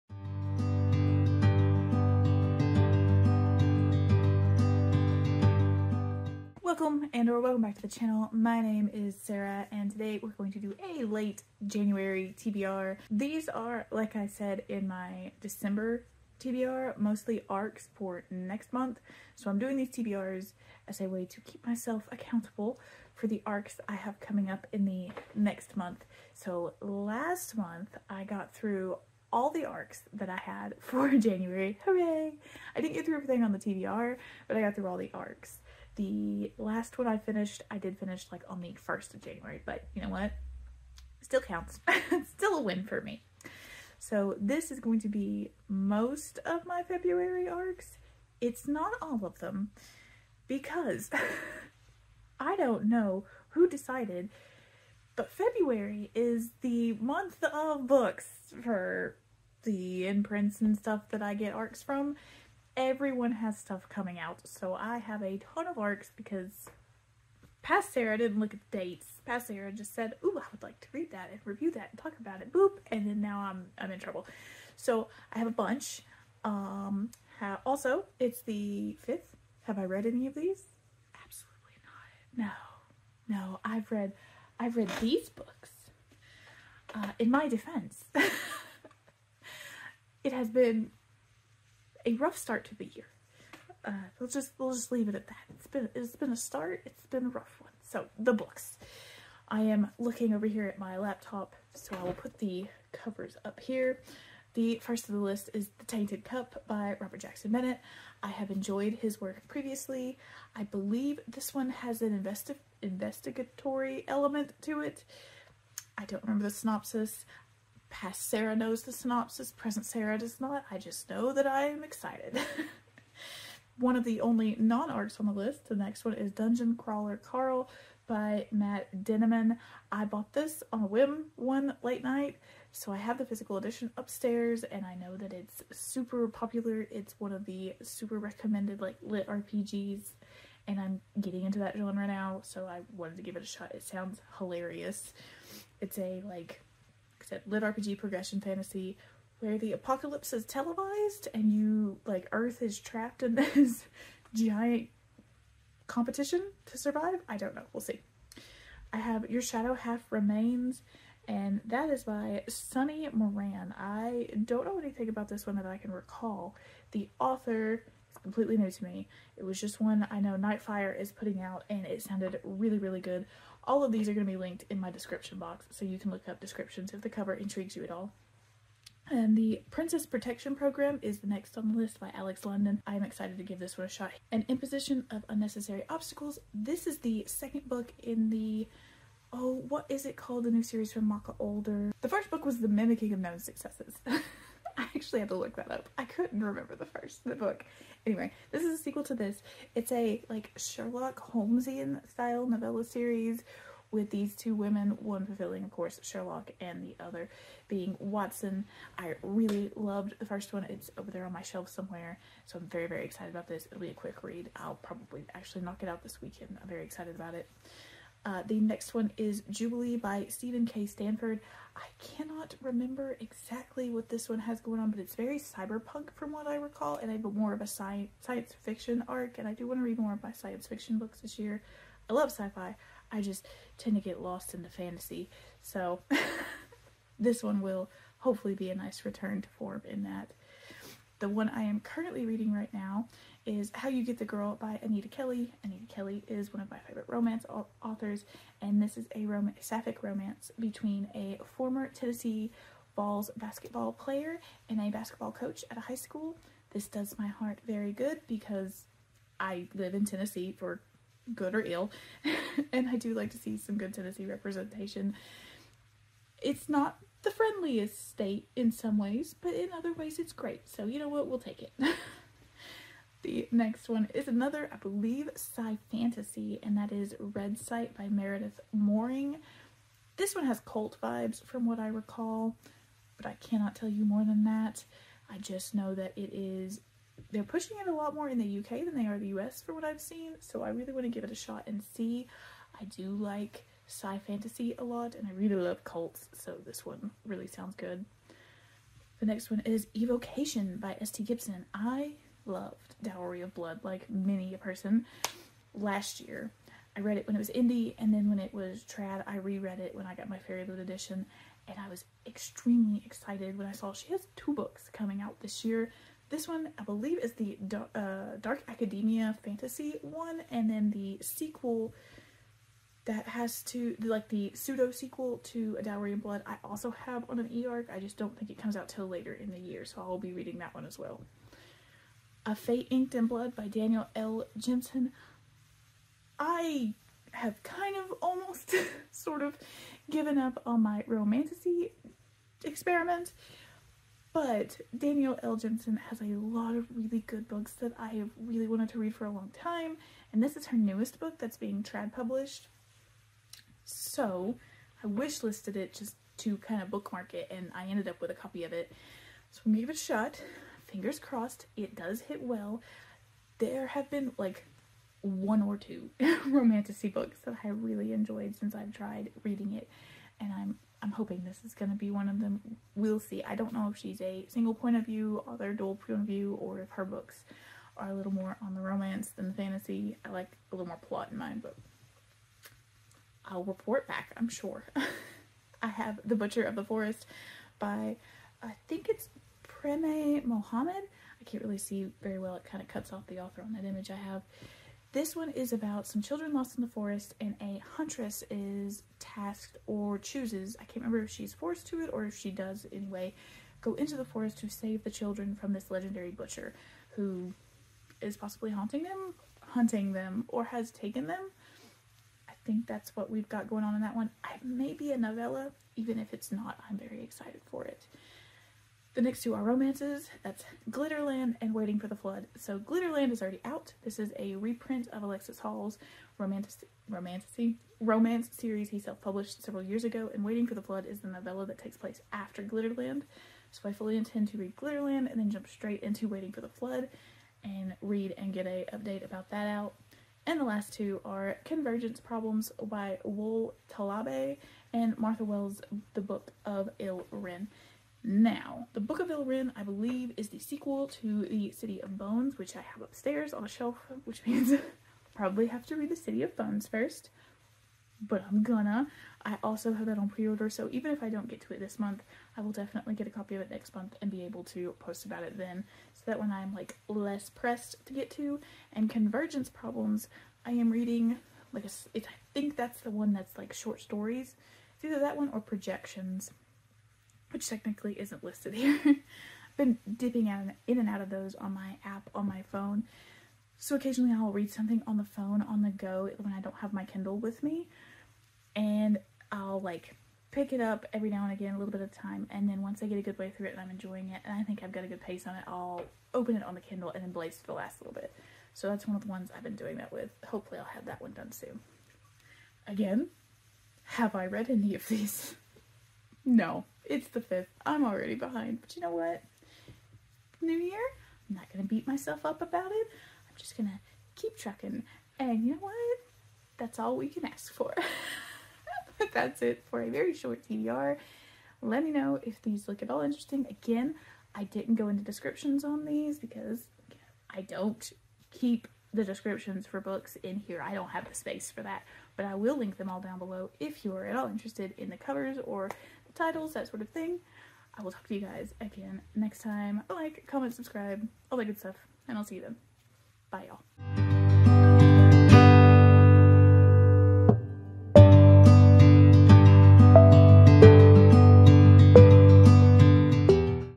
January TBR - Why is Everything Coming Out in Feb?
⭐Books mentioned⭐
- The Tainted Cup
- Dungeon Crawler Carl
- Your Shadow Half Remains
- The Princess Protection Program
- The Imposition of Unnecessaries Obstacles
- How You Get The Girl
- Fate Inked in Blood
- The Butcher of the Forest
- Waiting for the Flood
- Convergence Problems
- The Books of Ile-Rein

⭐Amazon Wishlist⭐